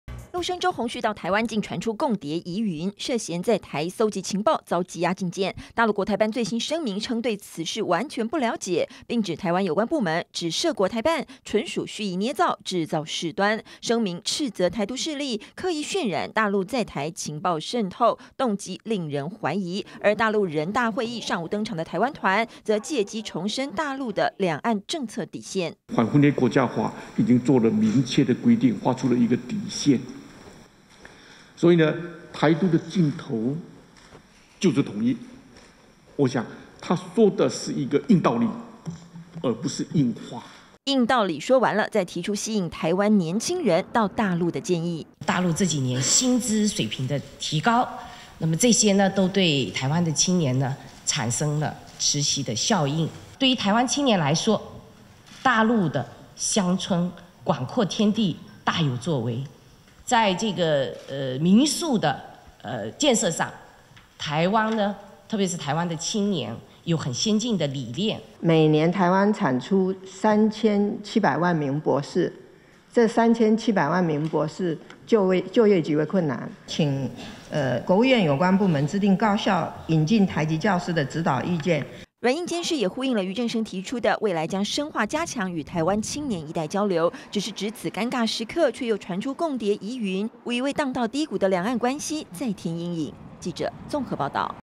20170310中天新聞 陸生涉共諜？ 陸國台辦：蓄意捏造
台北地檢署羈押一名涉嫌情搜的陸生，為兩岸關係蒙上陰影，大陸國台辦隨後發表聲明，稱對此事完全不了解，同時指責是台獨勢力，在刻意渲染大陸在台情報工作。